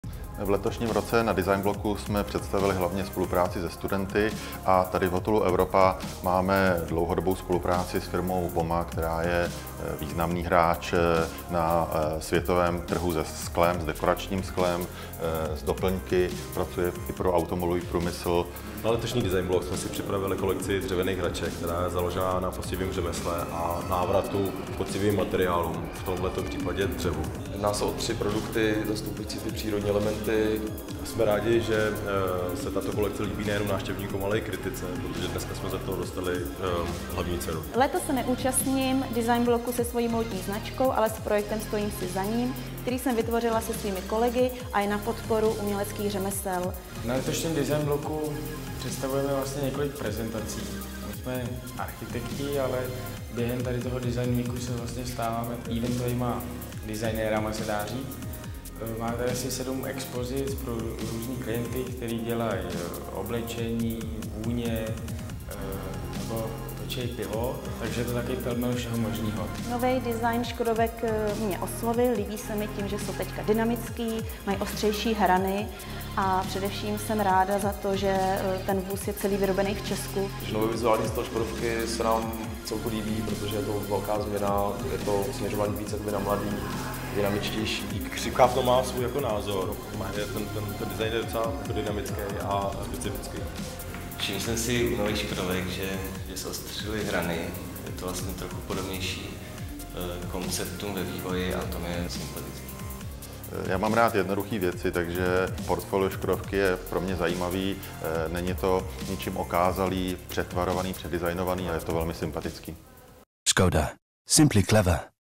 Designéři na Designbloku 2014
Letošní Designblok opět patří mezi největší designové show v Evropě. Jeho expozice naleznete celkem na 65 místech v Praze, vystavovatelé na Designblok letos přijeli z celkem 15 zemí z celého světa! Je nám ctí představit vám ty nejzajímavější z nich a také jejich designové výtvory.

A jak tito designový znalci hodnotí nový designový jazyk značky ŠKODA? Jejich odpovědi najdete v tomto videu!